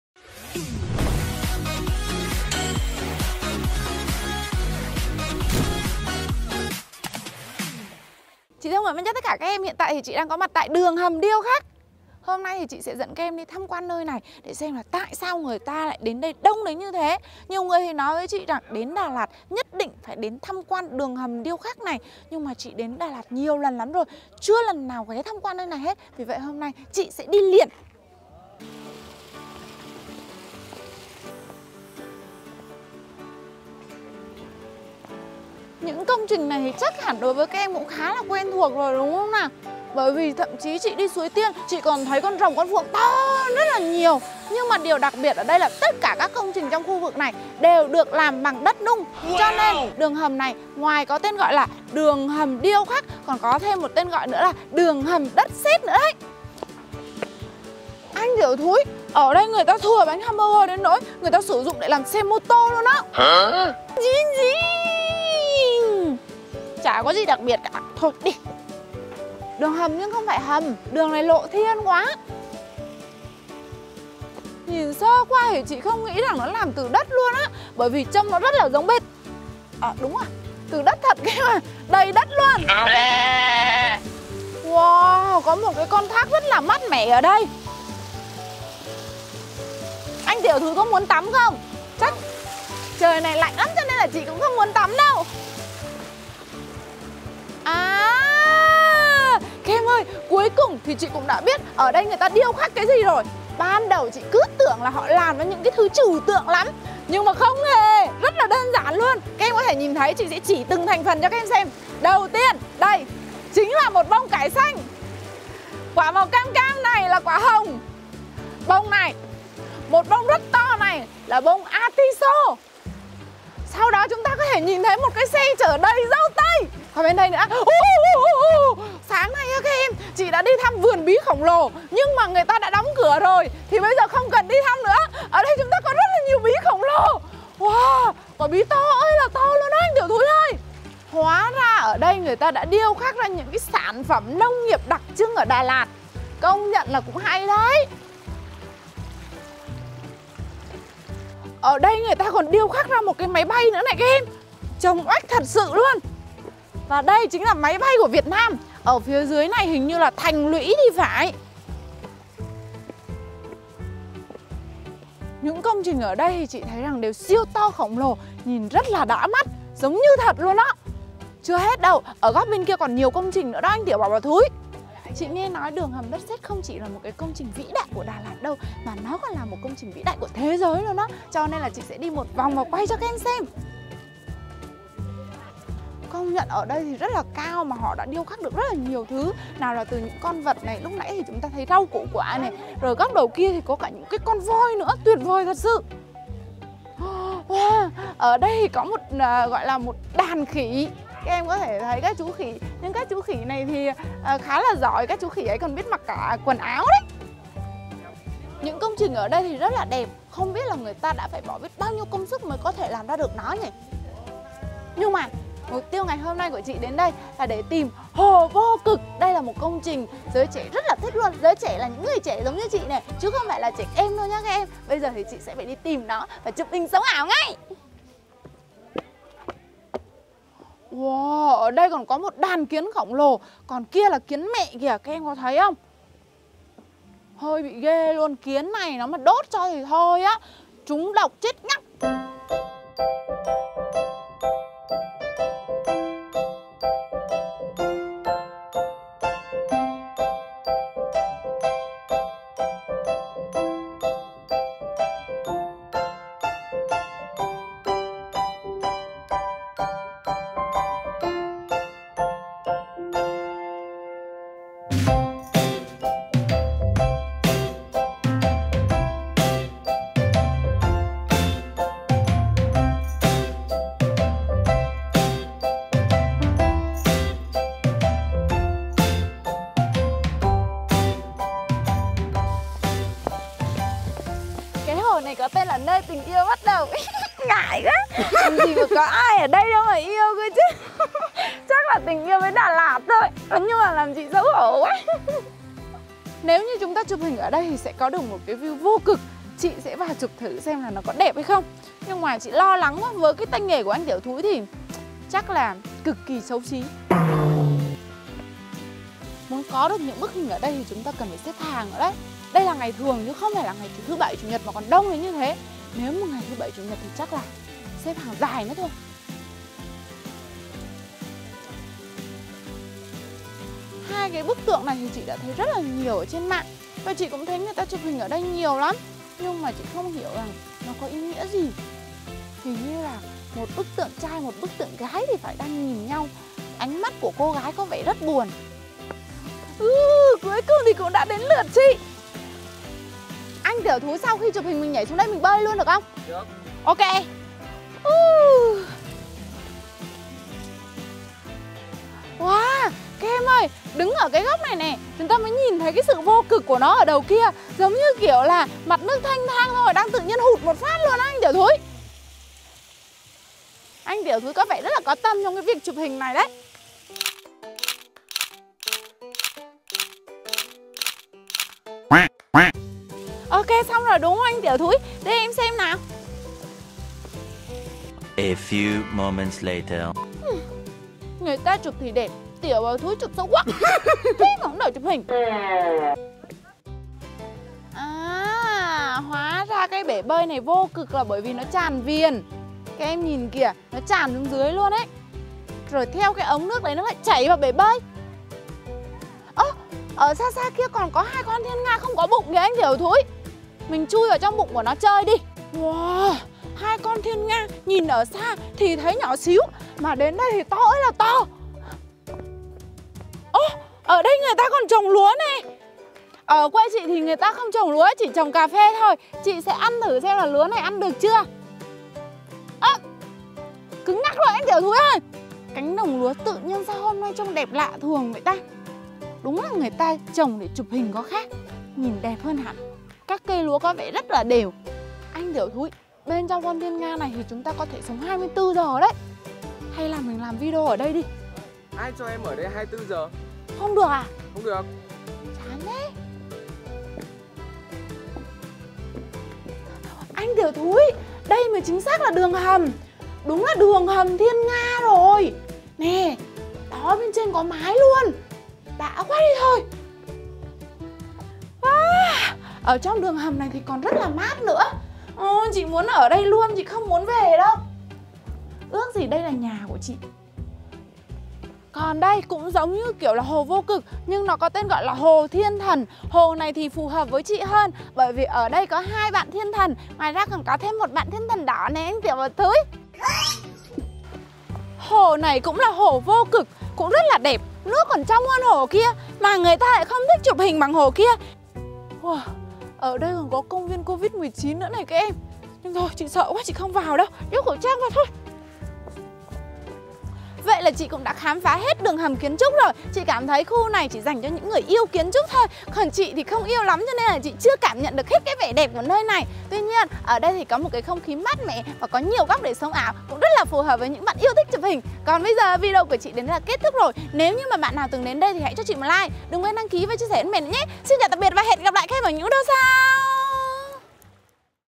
Đi Chơi Hồ Vô Cực Ở Đường Hầm Đất Sét
Hồ vô cực là 1 địa điểm cực hot của các bạn trẻ ở Đà Lạt từ lâu mà bây giờ chị mới biết.